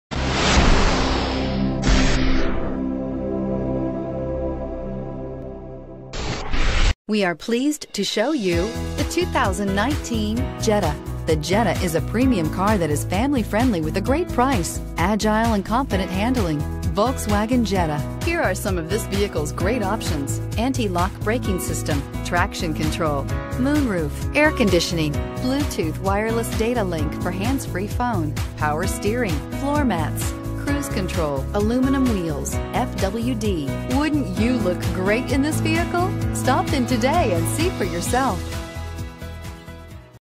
2019 Volkswagen Jetta Irvine, Santa Ana, Costa Mesa, San Juan Capistrano V190211
2019 Volkswagen Jetta 1.4T SE Pure White 2019 Volkswagen Jetta 1.4T SE FWD 8-Speed Automatic with Tiptronic 1.4L TSIbrbr30/40mpg Recent Arrival! 40/30 Highway/City MPG